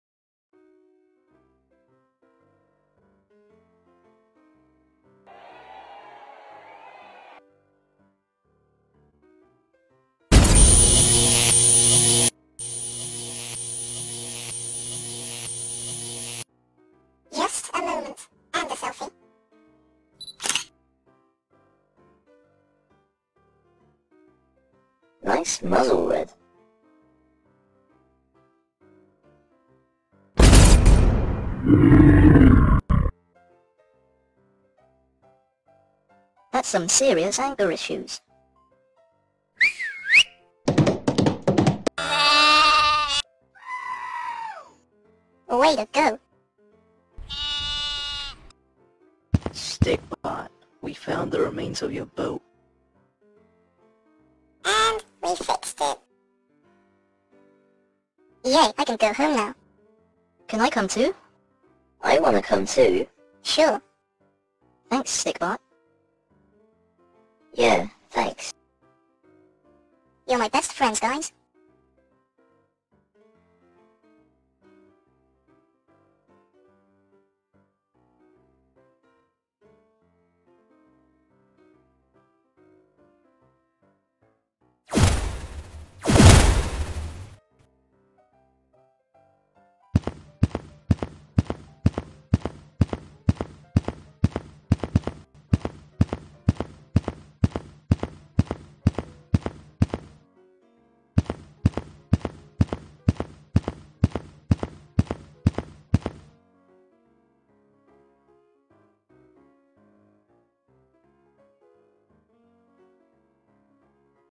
Ocean Survival💦🐟 ep.5
Red (from angry birds) got so angry he became a t-rex! Then a sheep scared him. Two random bots found Stikbot's boat and fixed it! Hey fanbots! What are your ideas for the next episode? Please write them in the comments below!